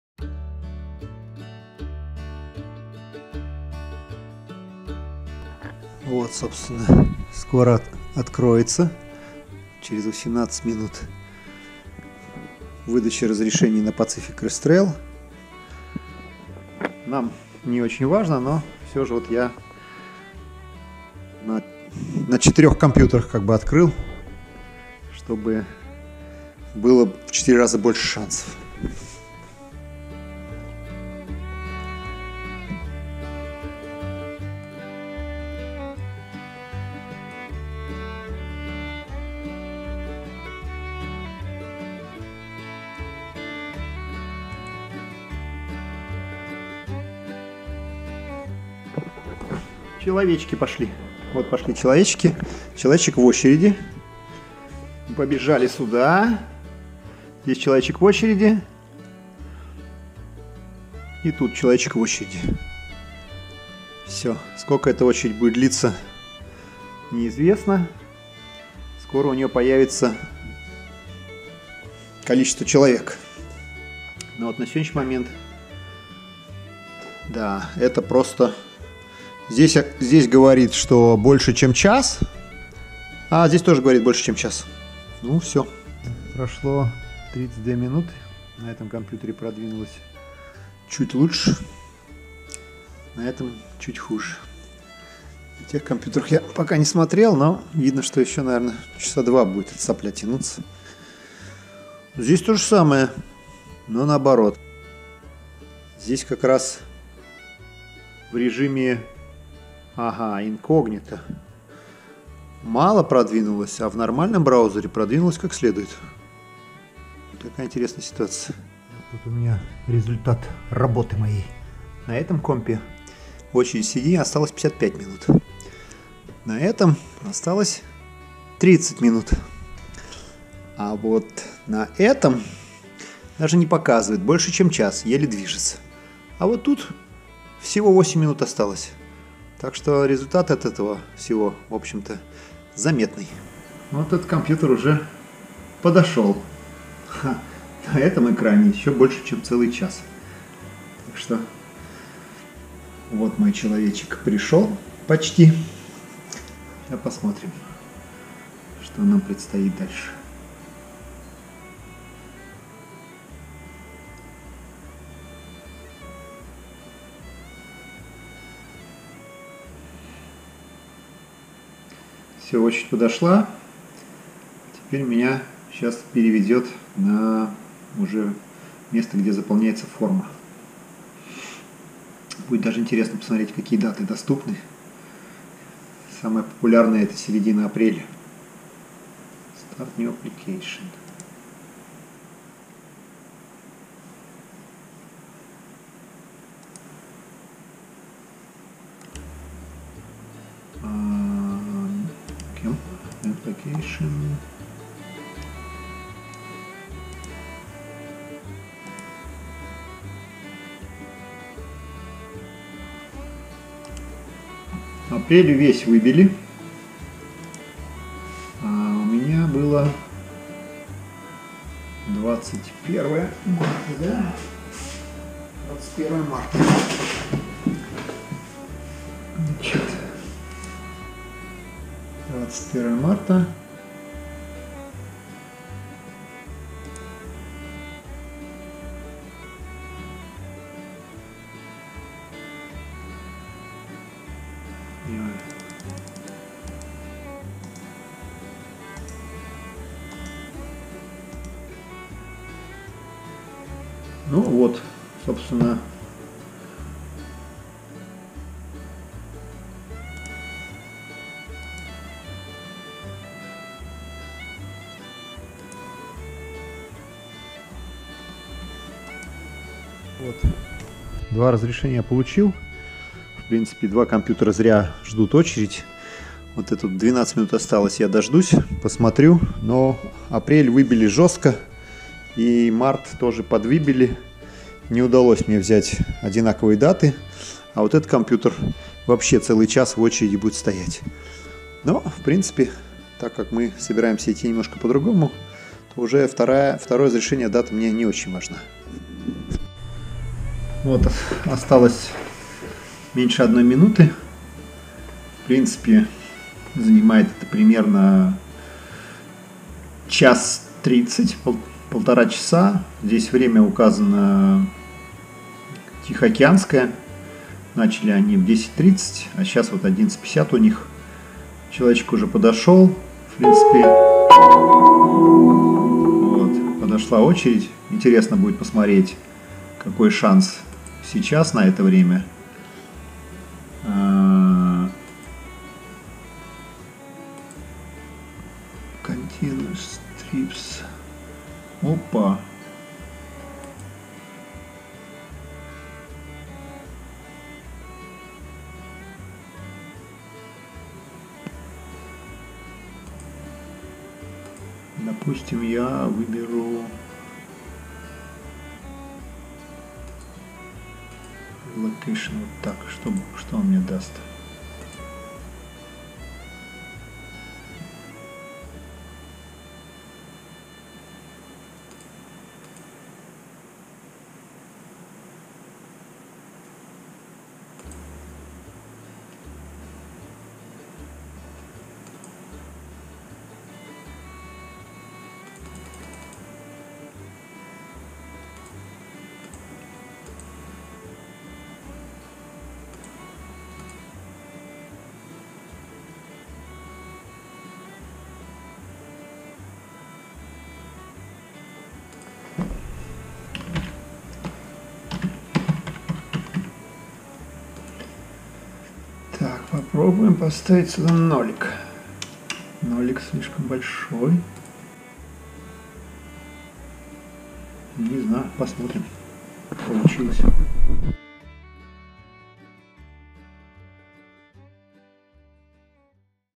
PCT permit 2022 , how to get better time slots. Как получить разрешение на PCT ( МТХ)
Как получить разрешение на PCT  ( МТХ) , показан сам процесс, и как его можно немножко модифицировать.  В последние 4-5 лет, выбиваются моментально все 50 места которые выдаются на один день,  к тому же изменили правила, и теперь нельзя ходить треил частями , который ты выбираешь сам ( Flip-Flop).  Из за сложности с лотереей на John Muir trail, многие стали брать секцию Sierra Nevada по тропе PTC, там 215 мили проходят совместно с John Muir trail.